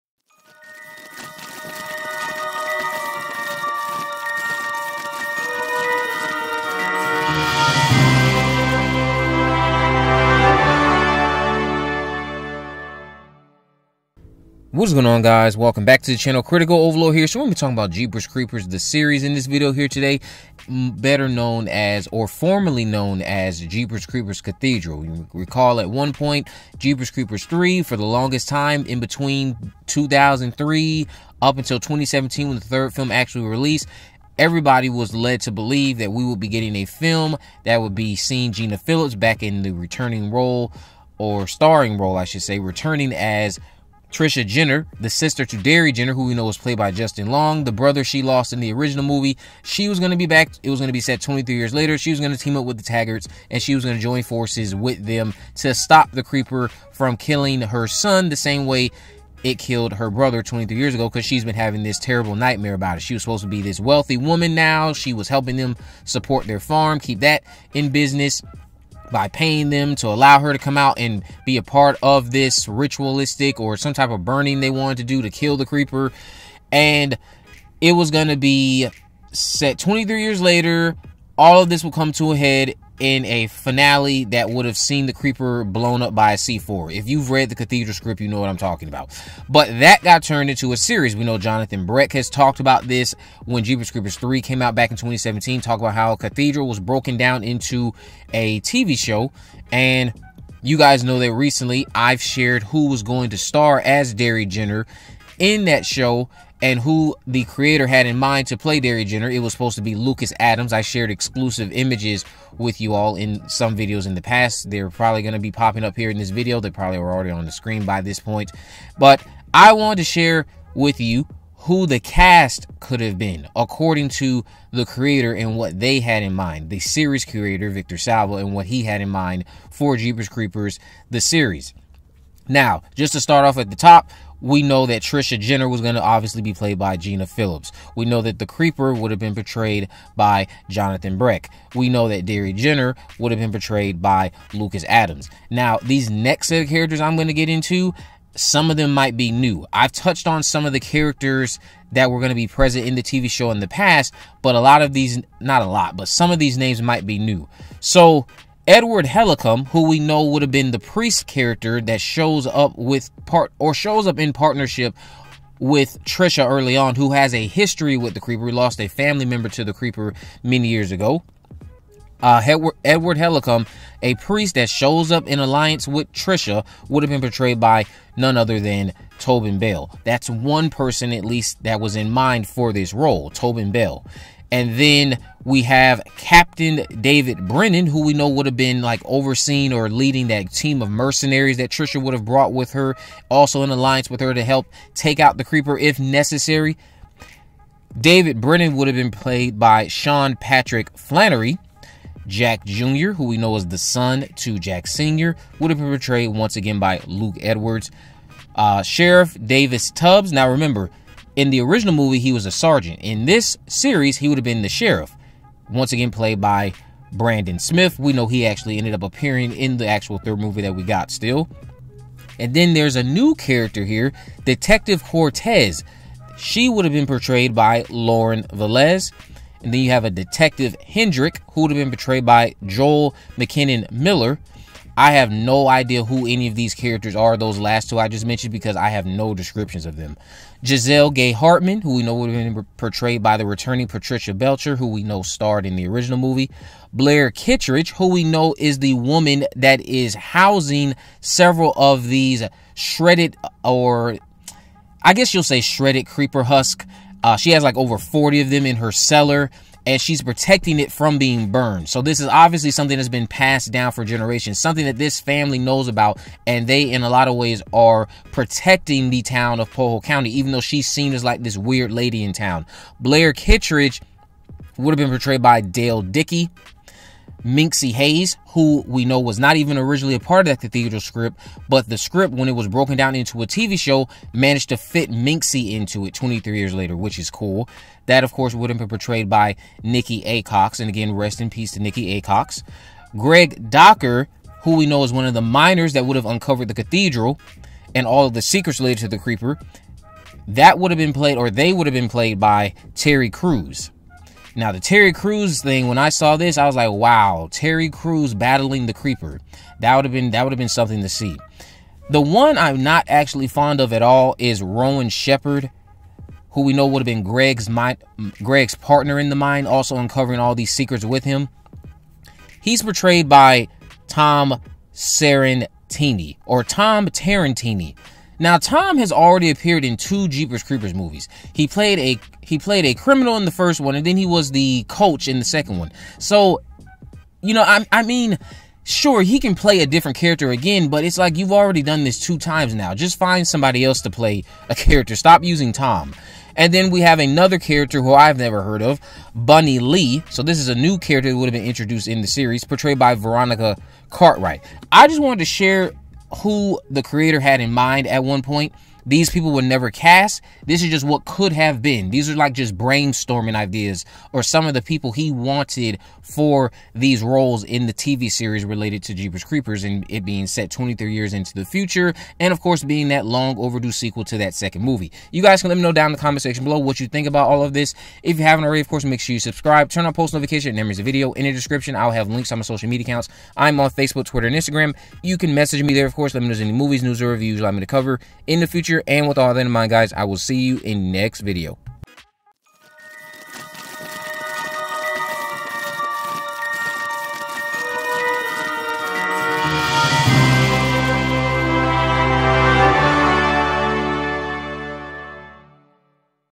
Jeepers Creepers | The Creeper Series Cast Plans Revealed
Jeepers Creepers: Reborn has come and gone with another lackluster Creeper movie. After a mostly negative reception, new interviews have arrived with the new Creeper director hinting that he might be very clueless and has no idea what is going on. The original Jeepers Creepers: Cathedral now exists as a TV show story, but who did the creator have in mind to join the scrapped series?